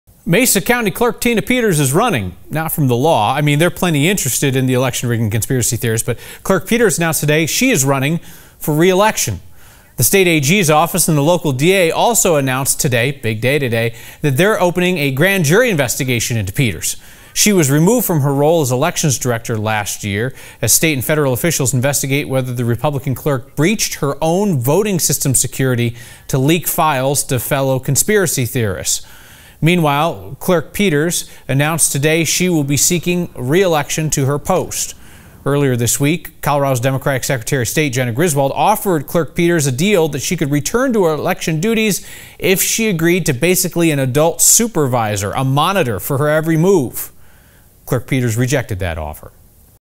Peters intends to run again for Mesa County clerk
Tina Peters said she will again run for clerk and recorder in Mesa County, as prosecutors announced a grand jury will investigate election equipment tampering.